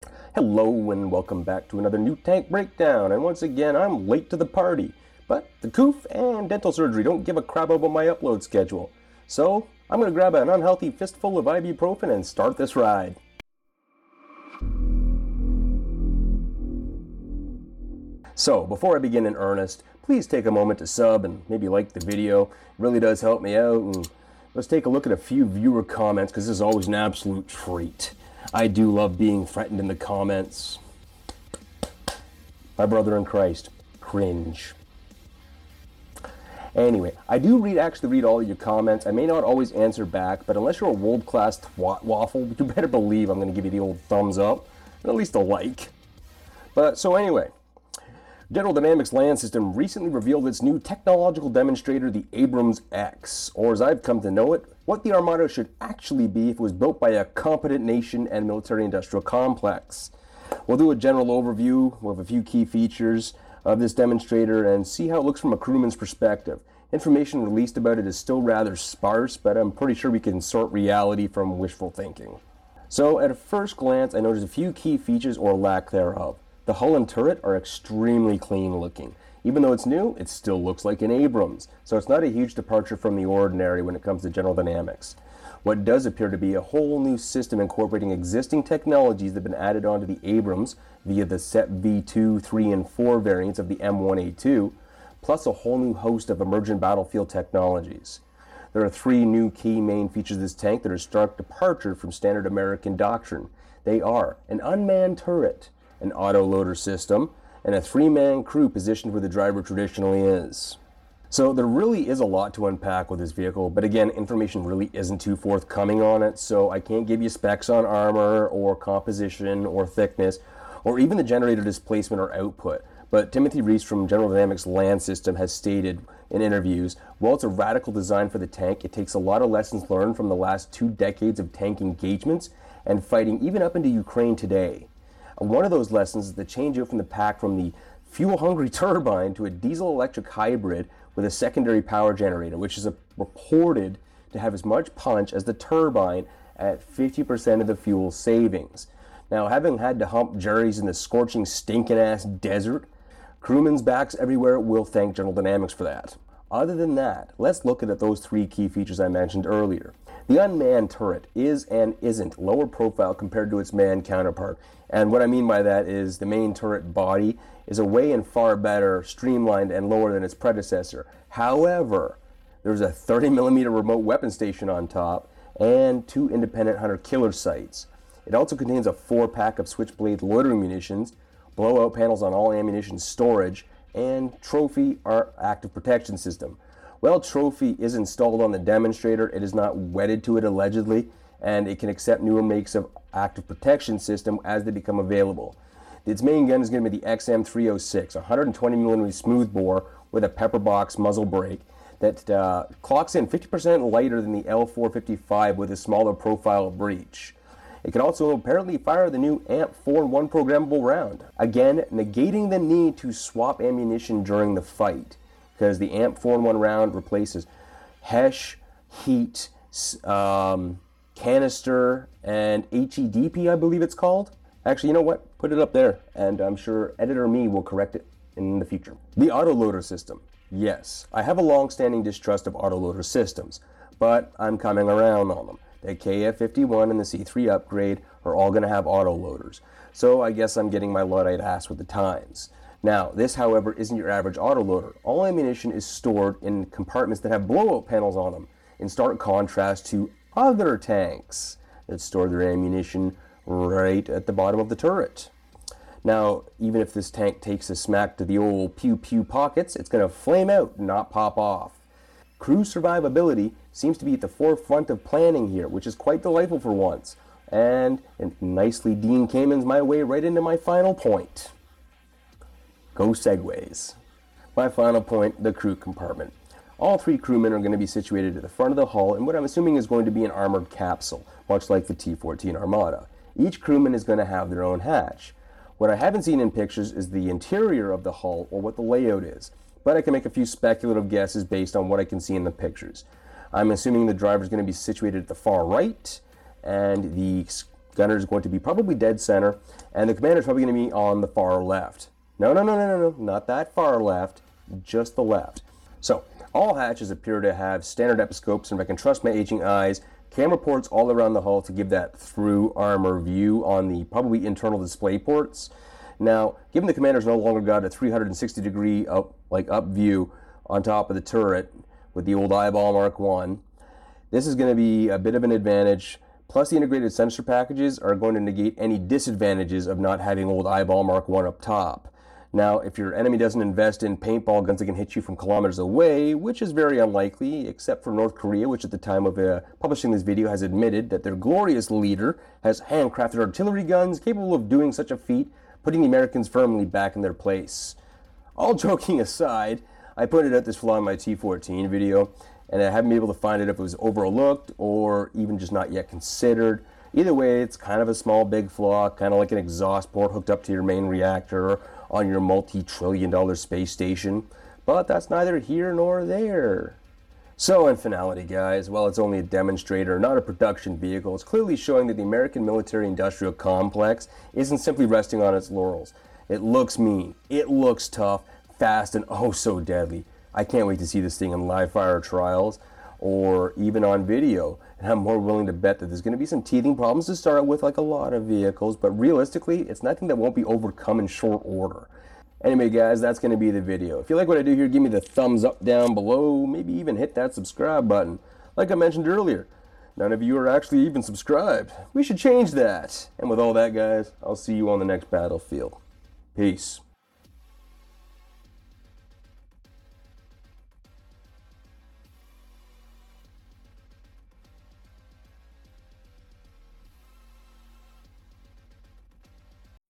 A Tankers View of the Abrams X
I finally get off my butt and join the party, better late than never. I look at the General Dynamic Land System Abrams X technology demonstrator from a simple crewmans perspective.

Video Clip Credit: General Dynamic land system; Northrop Grumman, Mil Tech 3-D.